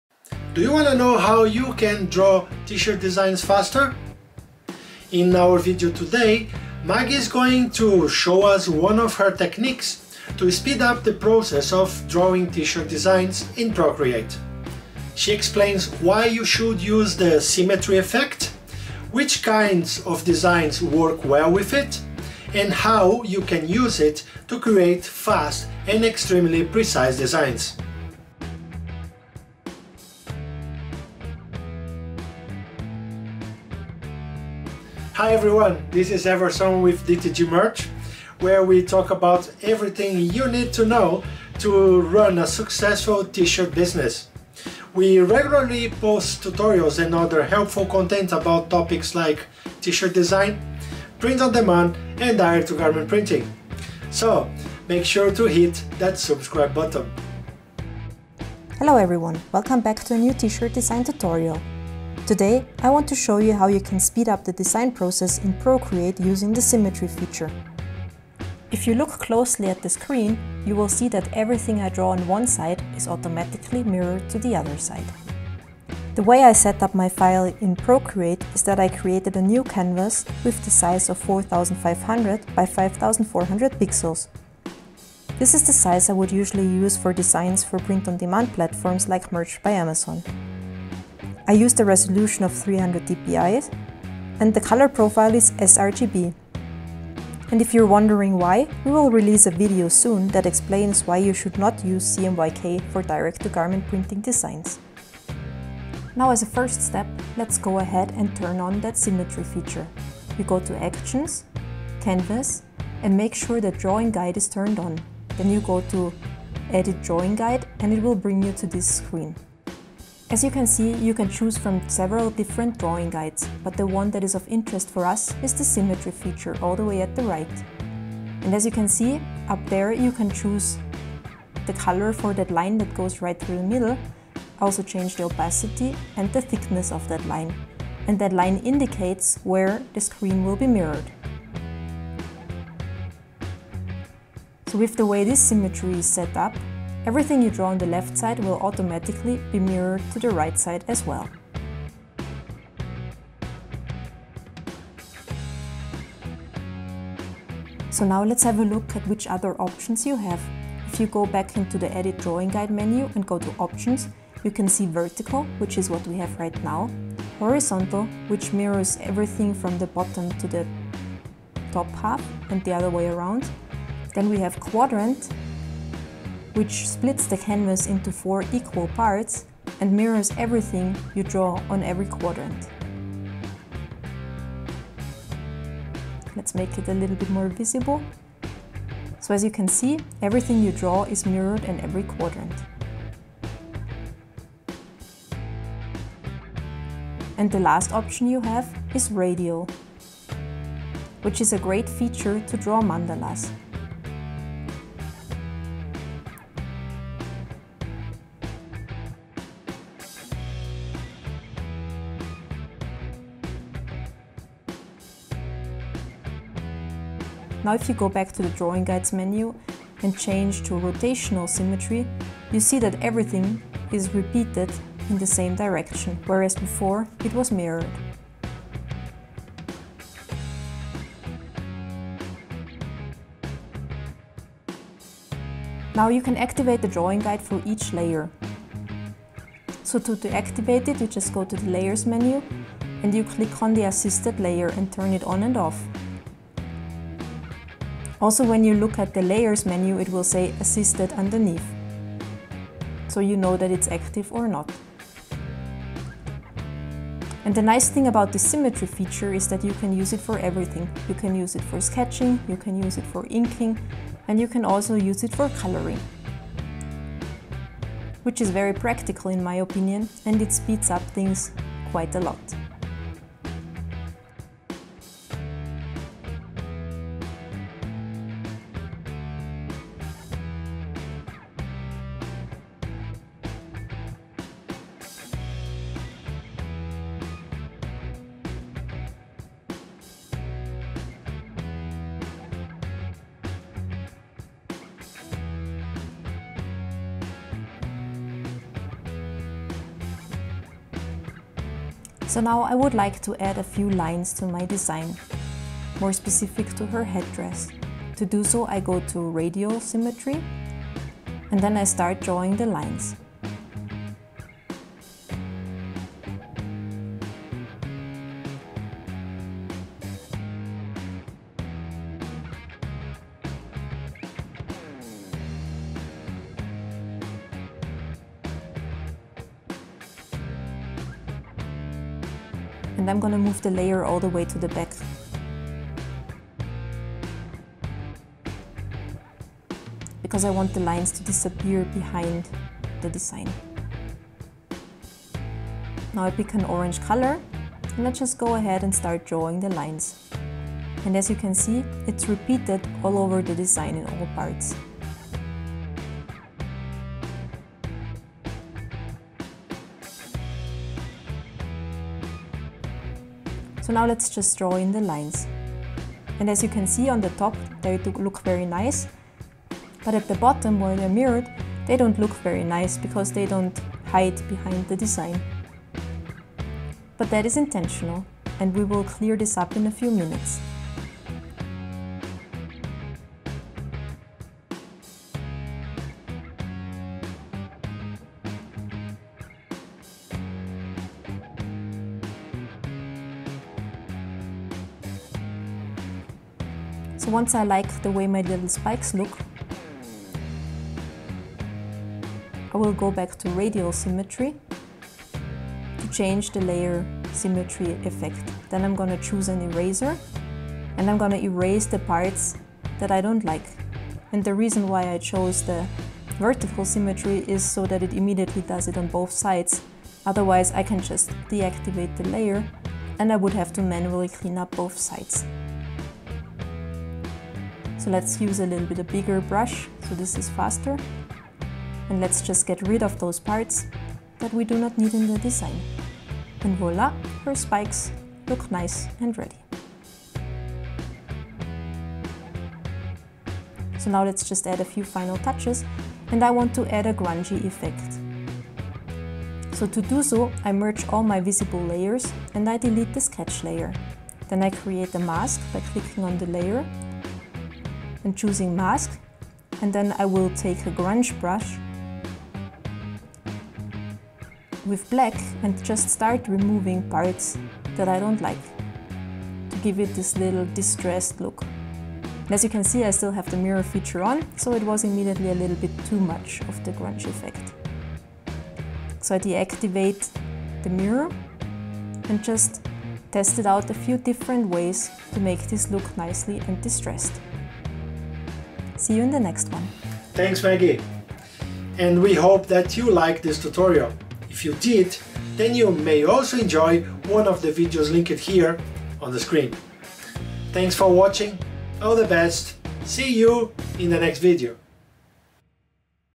Procreate T-Shirt Design Tutorial - Draw faster with Symmetry
In this Procreate T-Shirt Design Tutorial, we show how you can use the symmetry tool to design T Shirts faster. Since Halloween is around the corner, we creating a Dia de Los Muertos T-Shirt design.
This can be used for all Shirt designs, not only for direct to garment printing and print on demand. Don't forget: If you have any questions, please post them in the comments!